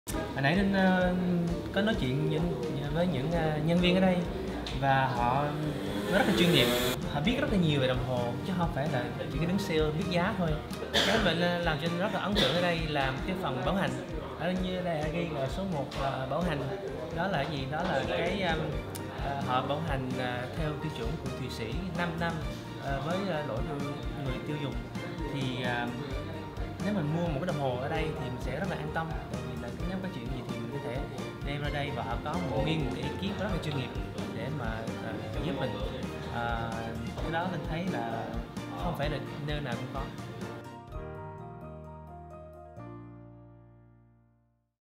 Nhạc sỹ Dương Khắc Linh đã tin tưởng Xwatch - Xchannel
Điều mà Linh ấn tượng nhất là chế độ bảo hành 5 năm theo tiêu chuẩn Thuỵ Sĩ cho cả lỗi người dùng. Điều này không phải nơi nào cũng có.

Hơn nữa khi đến XWatch, Linh còn được chia sẻ những câu chuyện rất thú vị về mỗi chiếc đồng hồ từ nhân viên bán hàng.

Rõ ràng các bạn không chỉ là kinh doanh đơn thuần. Các bạn mang cả tình yêu của mình vào từng sản phẩm.

Tin rằng XWatch sẽ còn phát triển hơn nữa trong tương lai!

Nhạc sĩ DƯƠNG KHẮC LINH